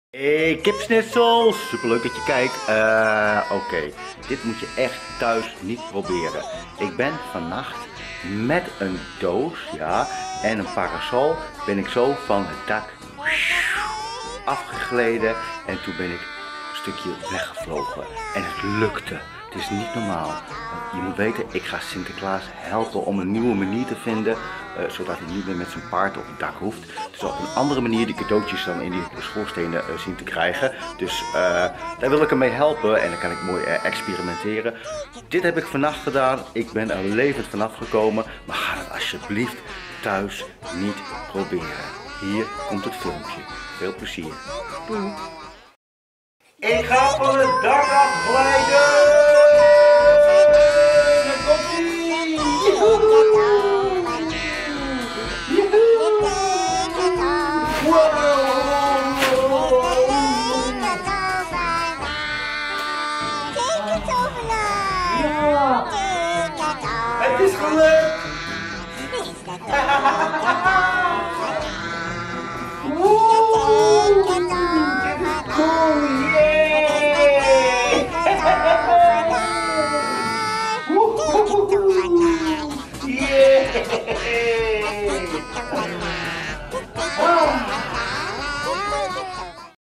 Niet thuis proberen! | Met een doos en parasol 's nachts het dak af! | Liedjes, muziek, vlogs
Niet thuis proberen! | Met een doos en parasol het dak af glijden! | Liedjes, muziek, vlogs
DE TEKENTOVENAAR! Lach je stuk en maak dikke lol!
Liedjes, Muziek, Tekenles, schilderles, muziekles, creatief denken en meer!